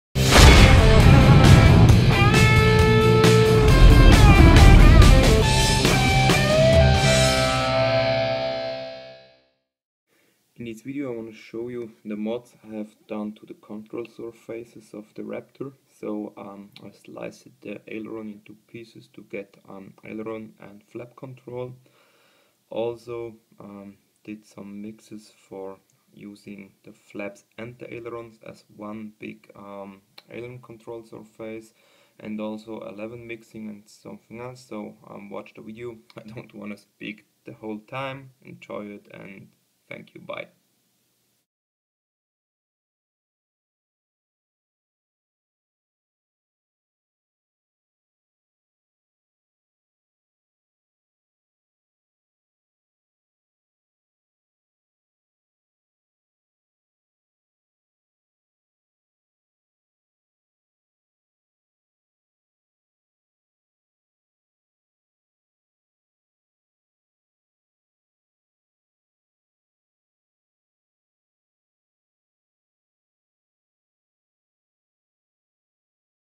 F-22 Raptor 70mm (Starmax/Hobbyking): 2. Control Surfaces and Mod
Short Video about the Control Surfeces Mod to the Ailerons and Elevons Mixing